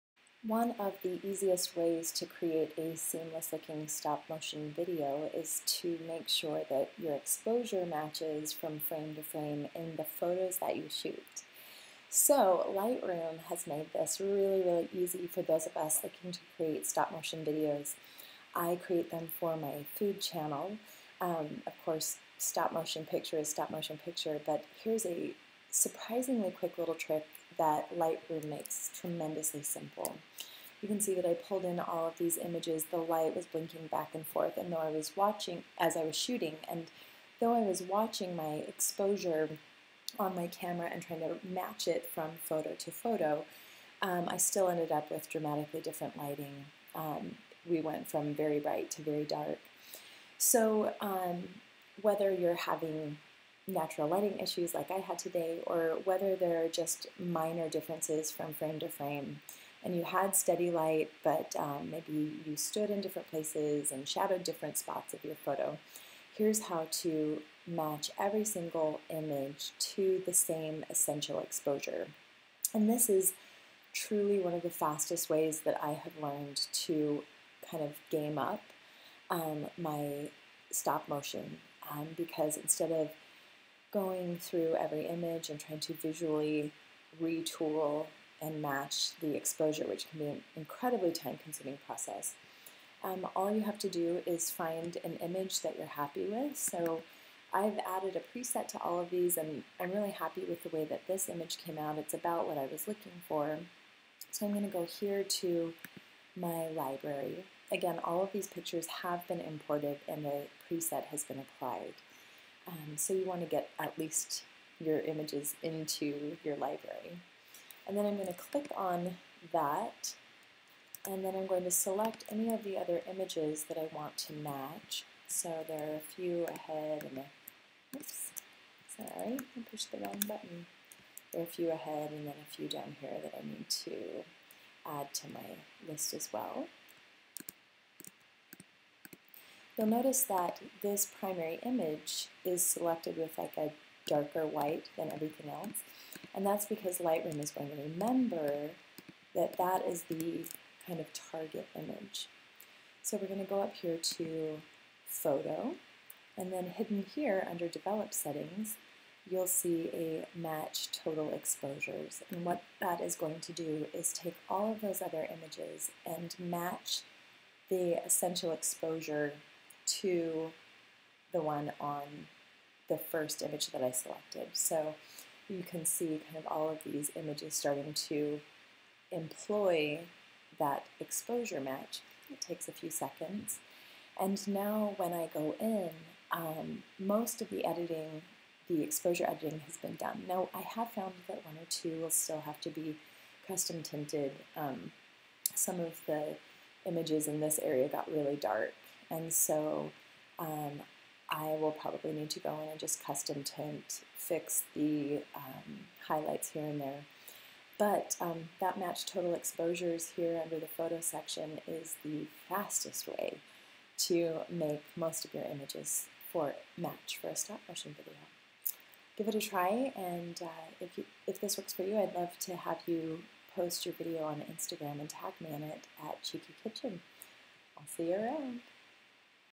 Reduce Stopmotion Flicker: Match Exposures on Photos in Lightroom
One of the simplest ways to create quality stopmotion videos is to ensure every photo is similarly lit and edited. This one-click trick makes it easy to batch edit photos in Lightroom, so the exposure of each photo matches.
JOIN US! Come join me & thousands of foodie geeks in the Food Blogging, Photography & Videography Facebook group, where we share secrets daily.